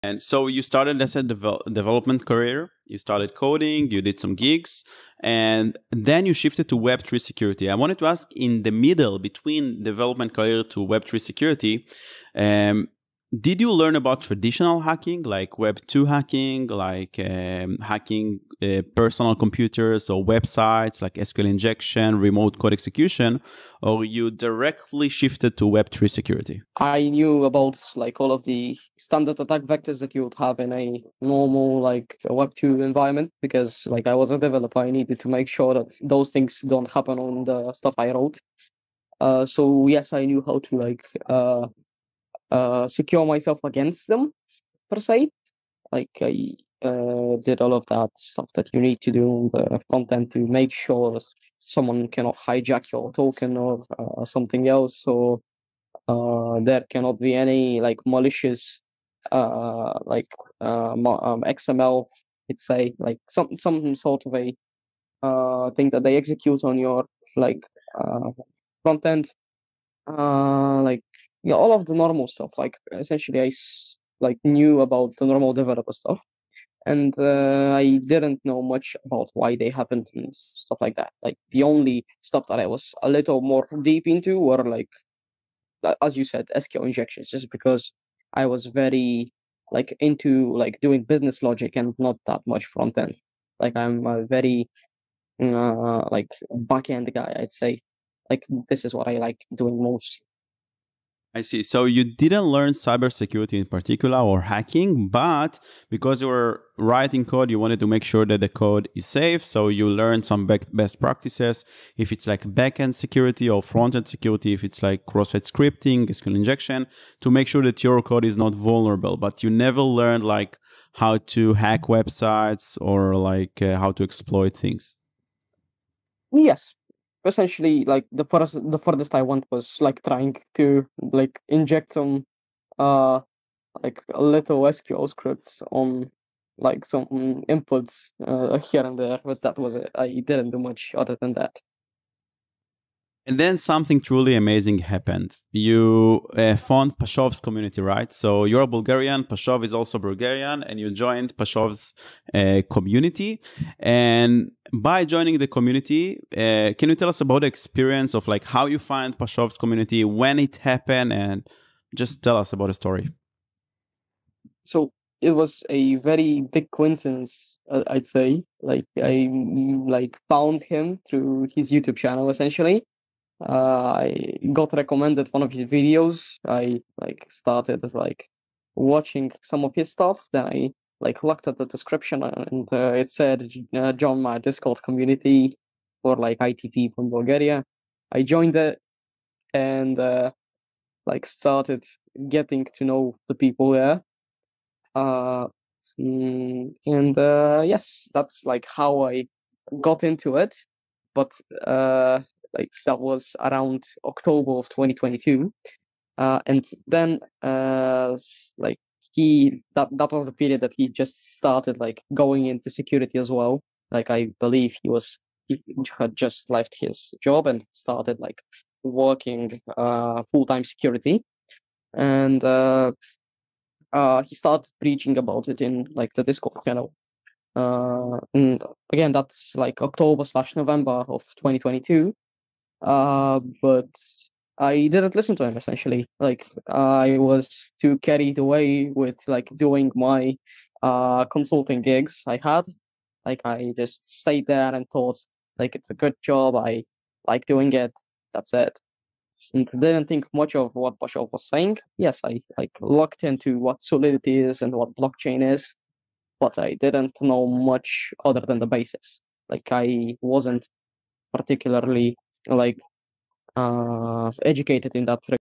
From Traditional to Blockchain Cybersecurity
In this video, Chriss shares more on his journey from traditional coding to blockchain cybersecurity, his experiences and how he transitioned from a development career to focusing on web3 security.

Discover how Chriss learned about traditional hacking techniques like SQL injection while ensuring the safety of his code. Find out how he stumbled upon Pashov's Community, a Discord group, and the interesting story behind it.

Watch the video to gain insights into Chriss' personal growth and his evolving understanding of blockchain and security.